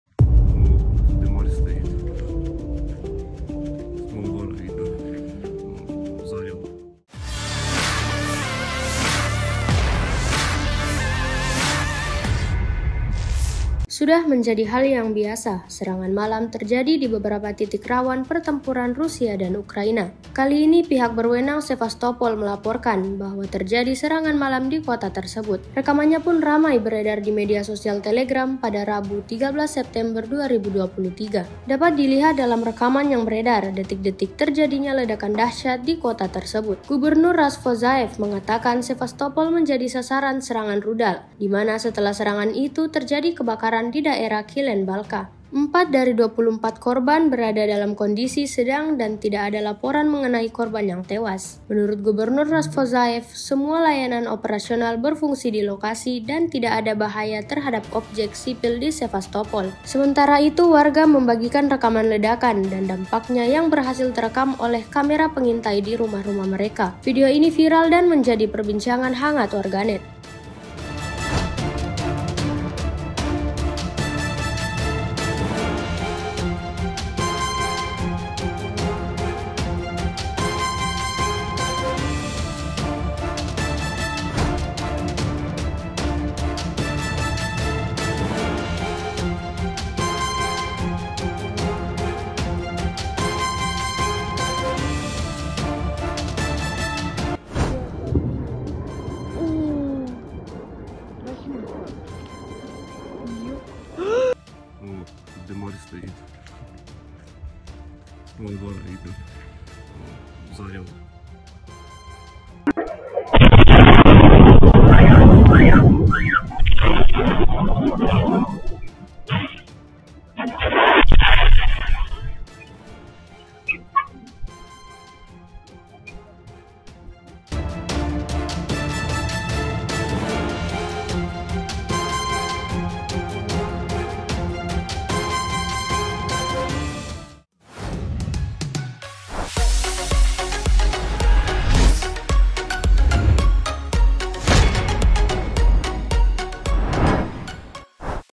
Serangan Malam di Sevastopol Gegerkan Warga, Ada Banyak Korban Jiwa?
TRIBUN-TIMUR.COM - Sudah menjadi hal yang biasa serangan malam terjadi di beberapa titik rawan pertempuran Rusia dan Ukraina.

Kali ini, pihak berwenang Sevastopol melaporkan bahwa terjadi serangan malam di kota tersebut.

Dapat dilihat dalam rekaman yang beredar detik-detik terjadinya ledakan dahsyat di kota tersebut.

Gubernur Razvozzhaev mengatakan Sevastopol menjadi sasaran serangan rudal, di mana setelah serangan itu terjadi kebakaran di daerah Kilen-balka.

Empat dari 24 korban berada dalam kondisi sedang dan tidak ada laporan mengenai korban yang tewas.

Menurut Gubernur Razvozhaev, semua layanan operasional berfungsi di lokasi dan tidak ada bahaya terhadap objek sipil di Sevastopol.

Sementara itu, warga membagikan rekaman ledakan dan dampaknya yang berhasil terekam oleh kamera pengintai di rumah-rumah mereka.

Video ini viral dan menjadi perbincangan hangat warganet.

Naskah: Memet Gunawan
Voice Over: Nurfadhilah Maulidiah (Mahasiswa Magang Unismuh)
VP: Asrul